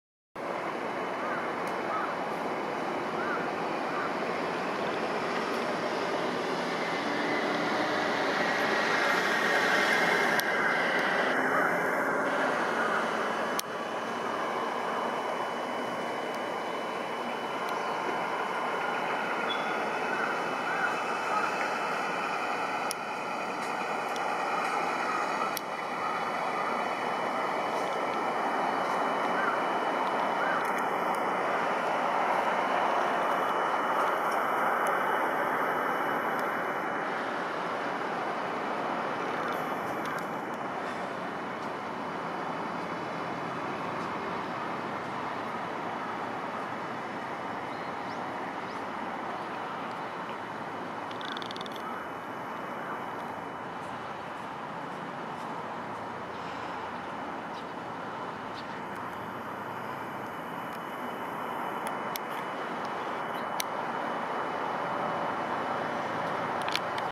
【Zoom Up】名古屋城の天守閣に輝く金シャチと【内部構築工】リニア中央新幹線名城非常口新設工事 2020.11.8
名古屋城の天守の上に輝く金シャチは、火災から城を守ることを願って置かれているそうですが、雄と雌があり、実は名古屋城の金シャチは、北側の雄と南側の雌があって大きさや形が違います。Zoom Up